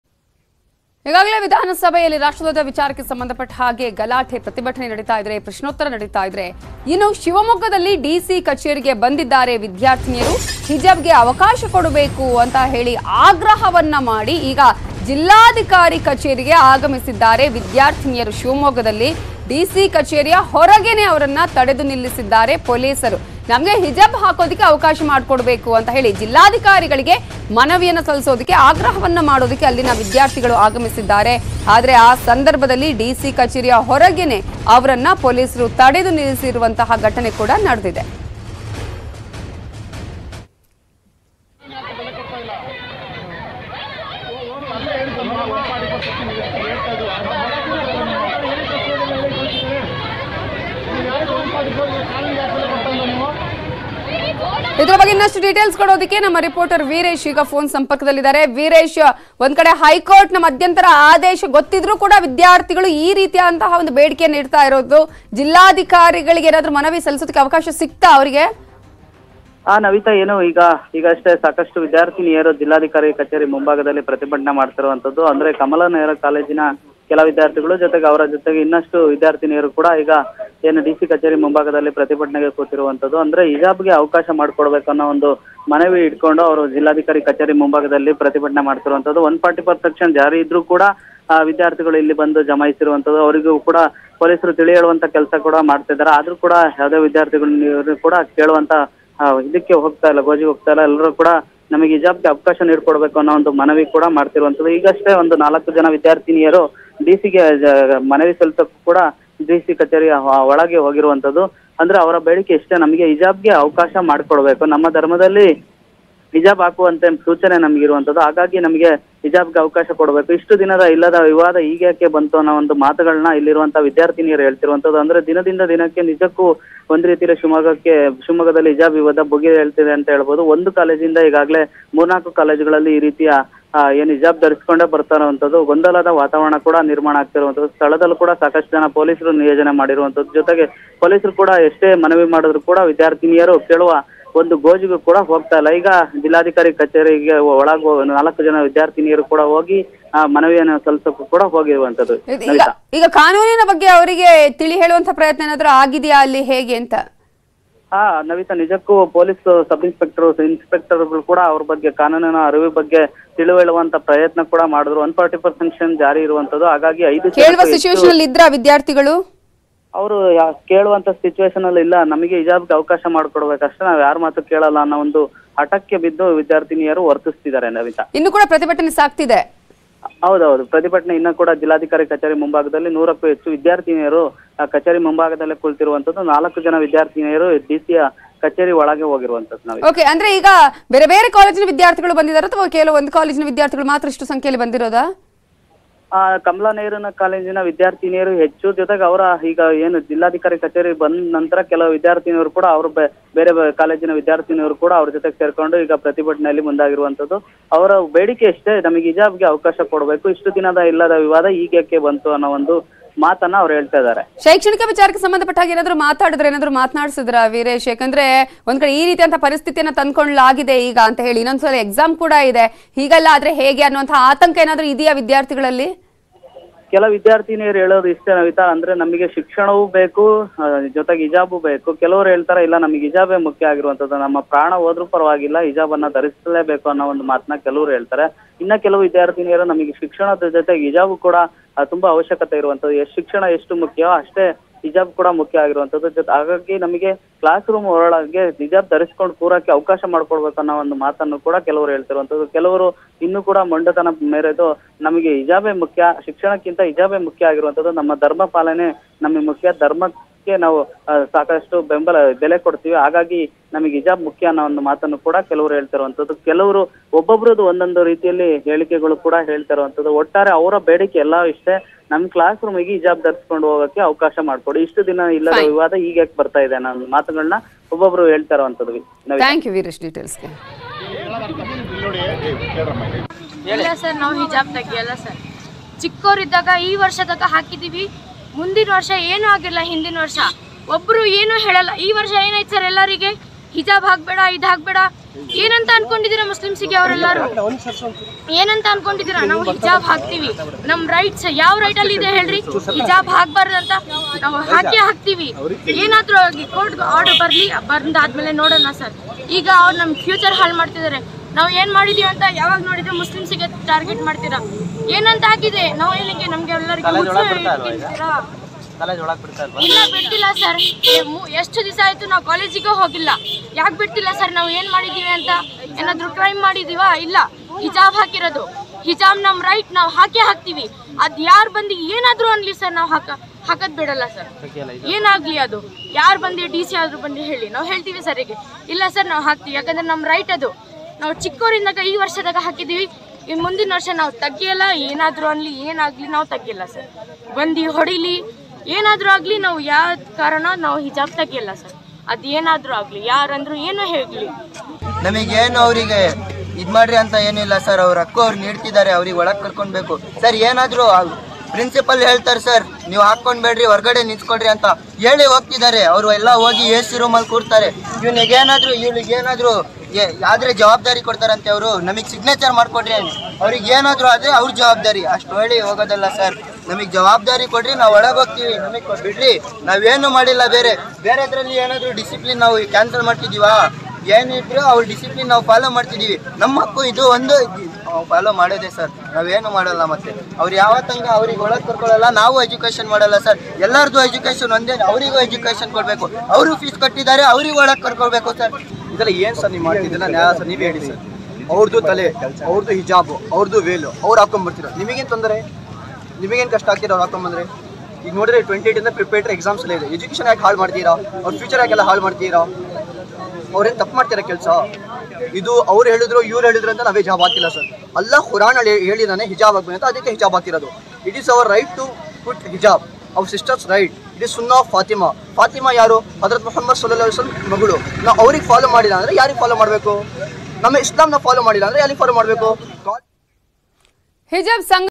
Hijab Controversy| Shivamoggaದಲ್ಲಿ DC ಕಚೇರಿಗೆ ಆಗಮಿಸಿದ ವಿದ್ಯಾರ್ಥಿನಿಯರು; ಹಿಜಬ್ ಗೆ ಅವಕಾಶ ನೀಡುವಂತೆ ಮನವಿ
Hijab Controversy| Shivamoggaದಲ್ಲಿ  ವಿದ್ಯಾರ್ಥಿನಿಯರು DC ಕಚೇರಿಗೆ ಆಗಮಿಸಿದ್ದಾರೆ. ಹಿಜಬ್ ಗೆ ಅವಕಾಶ ನೀಡುವಂತೆ ಮನವಿ ಮಾಡಿಕೊಂಡಿದ್ದಾರೆ.